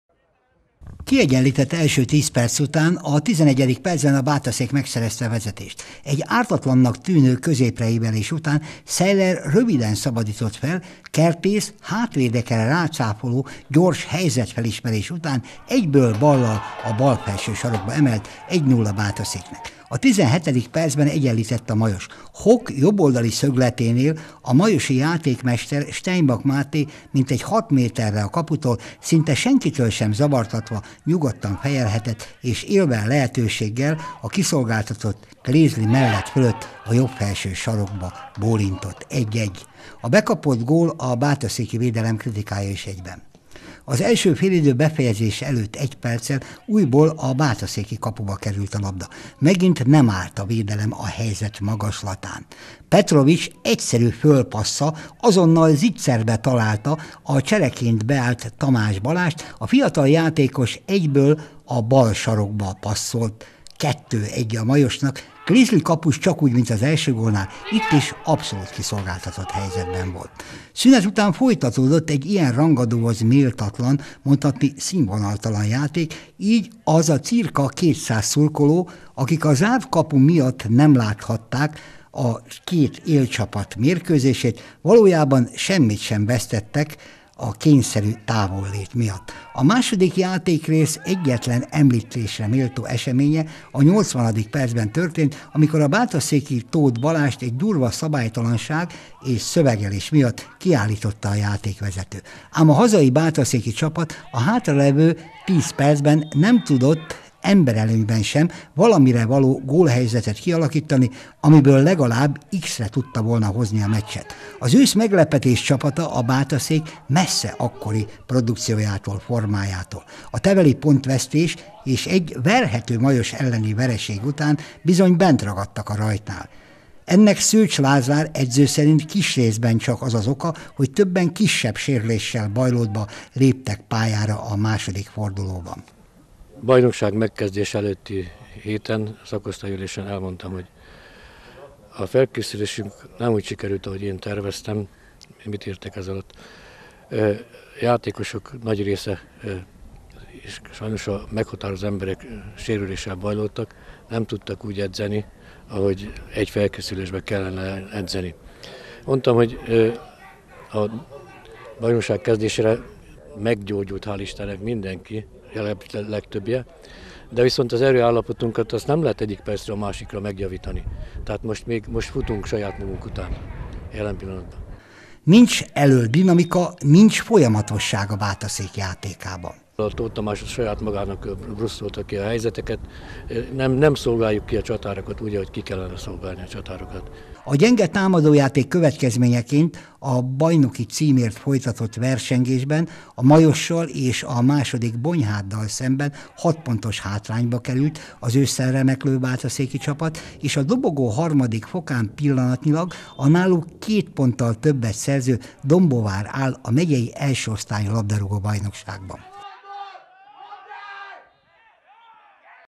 Bátaszék - Majos foci 20200324
A tavaszi, még lejátszott második fordulóban is megőrizte elsőségét a Majos, amely ismét egy élcsapattal, a Bátaszékkel találkozott, ezúttal idegenben. Elmaradtak a zöldek attól a produkciótól, amit tőlük megszokhattunk, ám a kapu előtti határozottság elég volt ahhoz, hogy három ponttal távozzanak a sérülésektől, formahanyatlásoktól sújtott Bátaszék otthonából.